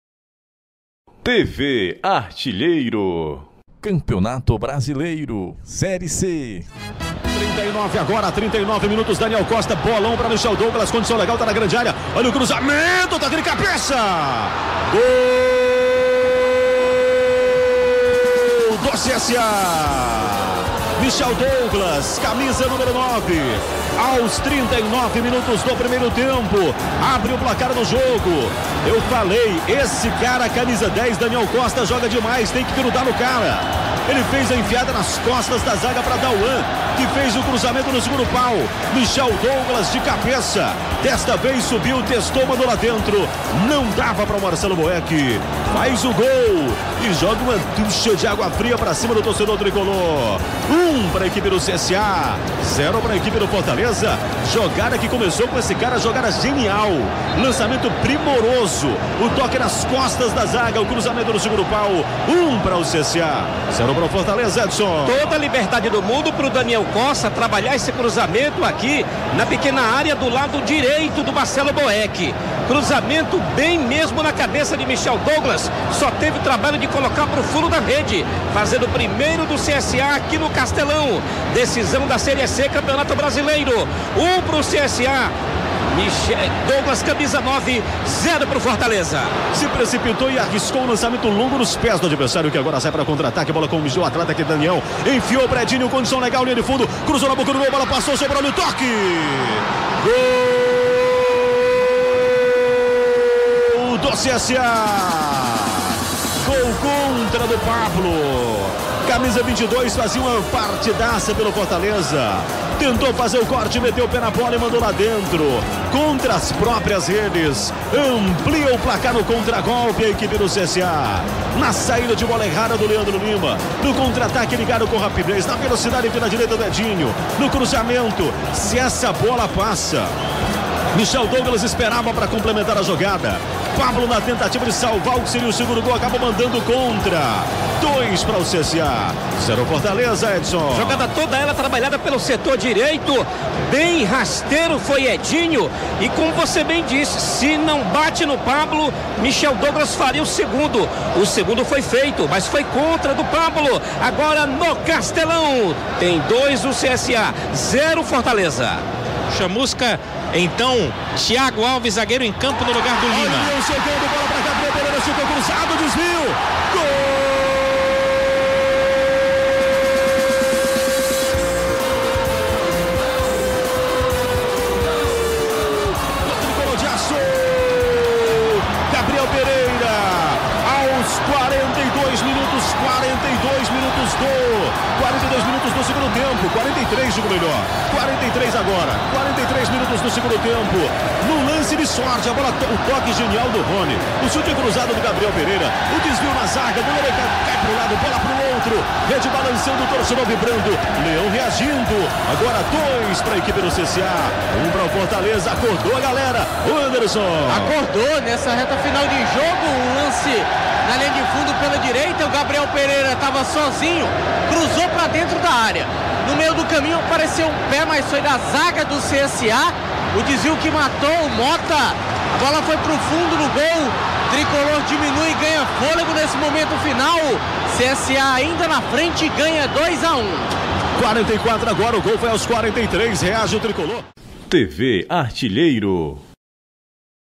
[Série C '17] Final 1 | Fortaleza EC 1 X 2 CSA/AL | Narração: Irismar França | TV ARTILHEIRO
[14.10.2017] Campeonato Brasileiro Série C | Final | 1ª Partida |
Fortaleza Esporte Clube 1 X 2 Centro Sportivo Alagoano |
Gols: Michel Douglas (39' 1T) e Pablo [Contra] (16' 2T) | CSA/AL |
Gabriel Pereira (42 2T) | Fortaleza EC |
Narração: Irismar França | Reportagens Fortaleza EC: Anderson Azevedo | Reportagens CSA: Edson Ferreira |
Rádio Tribuna Band News | FM 101.7 | Fortaleza/Ceará |